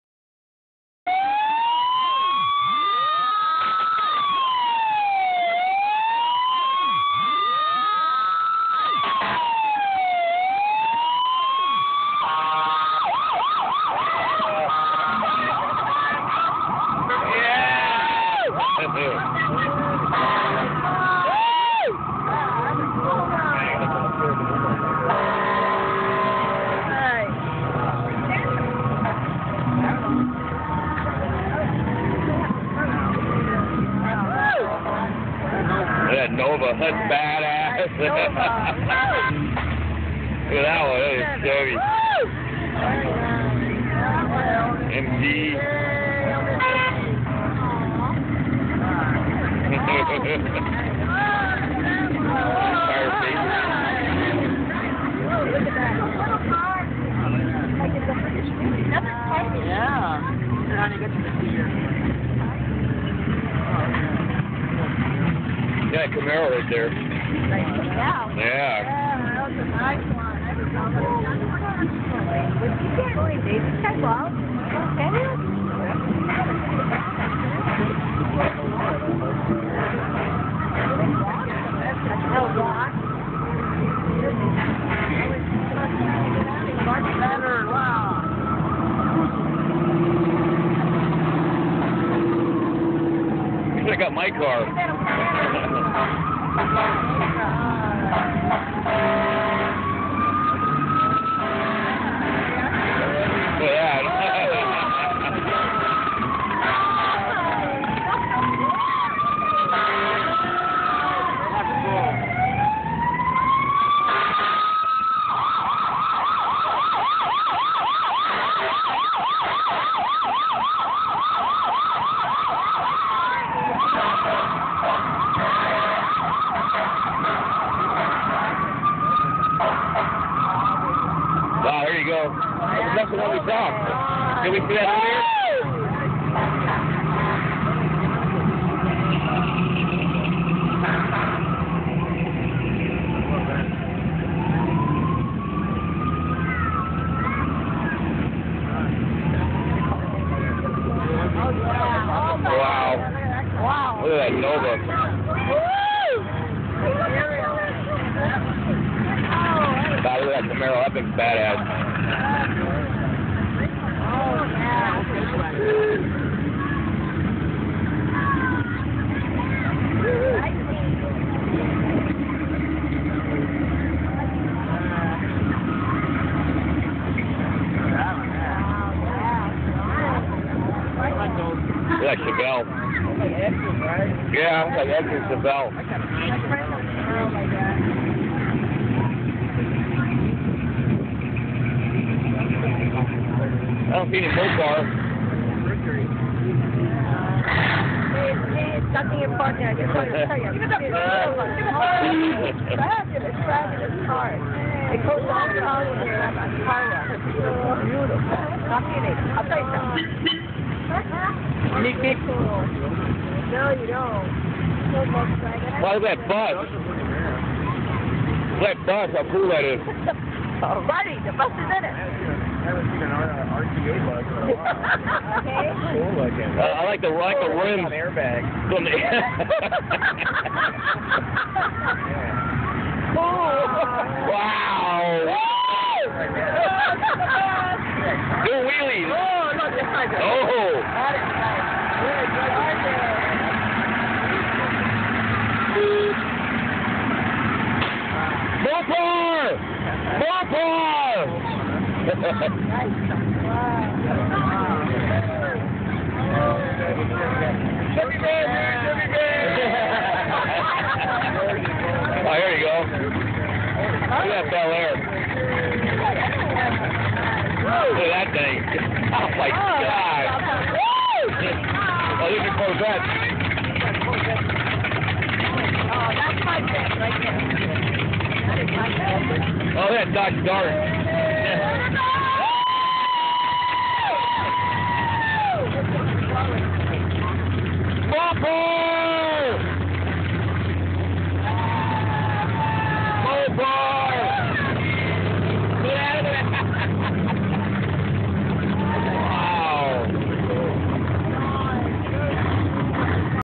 Arroyo Grande Annual Car Cruise and show
Car cruise and car show in Arroyo Grande, Ca.